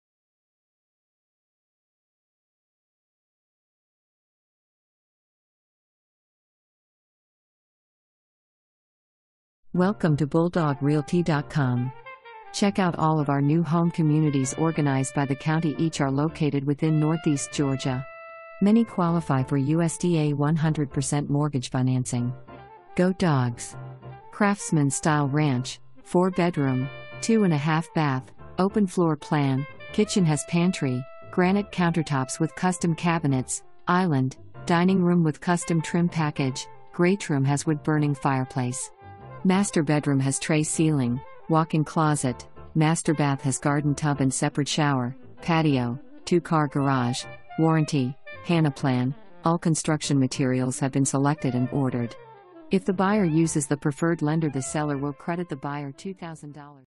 26 London Trail
26 London Trail
Winterville, GA 30683

Craftsman style ranch, 4 bedroom, 2 1/2 bath, open floor plan, kitchen has pantry, granite countertops with custom cabinets, island, dining room with custom trim package, greatroom has wood burning fireplace. Master bedroom has trey ceiling, walk in closet, master bath has garden tub and separate shower, patio, 2 car garage, warranty, Hannah plan, all construction materials have been selected and ordered. If the buyer uses the preferred lender the seller will credit the buyer $2000 at the time of closing. Photos are of the home plan being constructed, not of the actual subject property, features may vary.

2 full bath, 1 half bath

Charles Williams
BulldawgRealty.com